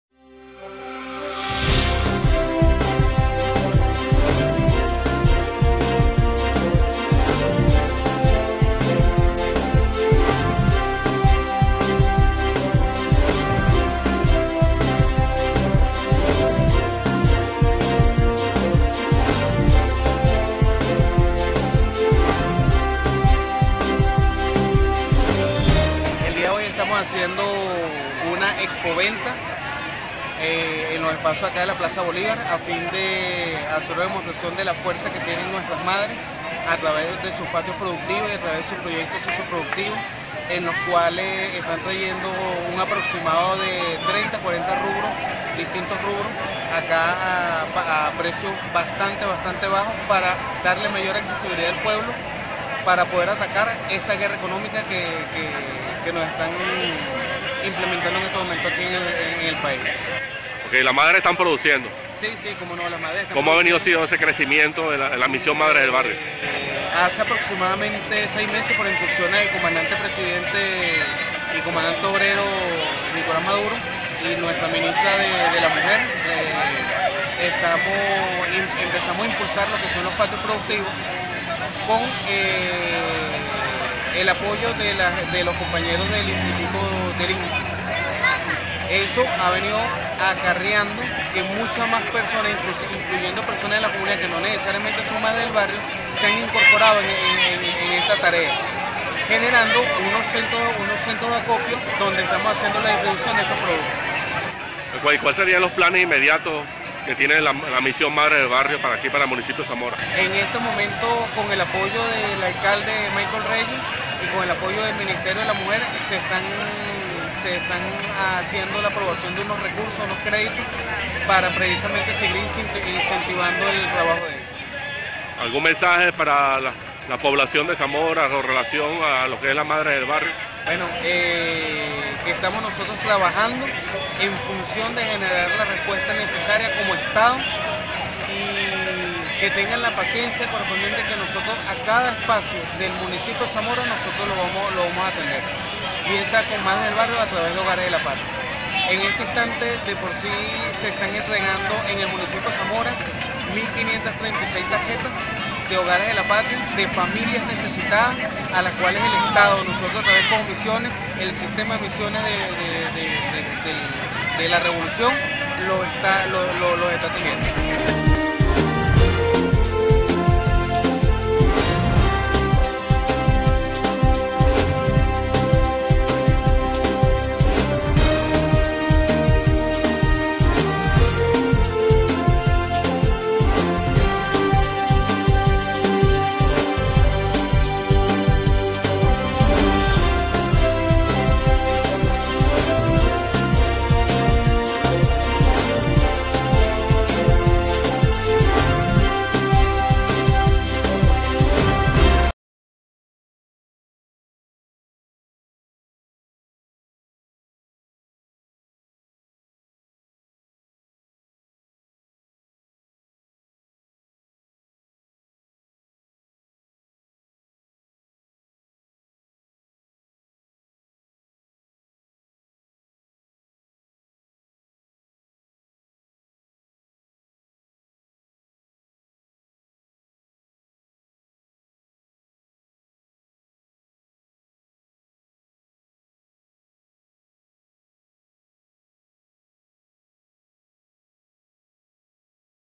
Madres de Barrio Productivas
(NotiComún-ZTV) Villa de Cura, 2/08/2016...
Misión Madres de Barrio continua la lucha por la transformación, en esta oportunidad las Madres del Municipio Zamora presentan sus logros en expoventa a la población villacurana.

Carlos Román, Coord. de la Misión Madres de Barrio de la localidad señaló que el propósito de la expoventa es demostrar la fuerza de las Madres de Barrio en la producción de alimentos y así combatir la guerra económica que viene enfrentando el país.

De igual forma, el Coordinador informó de la entrega de 1536 tarjetas de Hogares de la Patria, familias necesitadas, demostrando de esta forma como el Sistema de Misiones de la Revolución atiende a la población más vulnerable.

Producción Comunitaria
Miguel Ortiz
Reportero Comunitario
Equipo de NotiComún de Zamora Televisión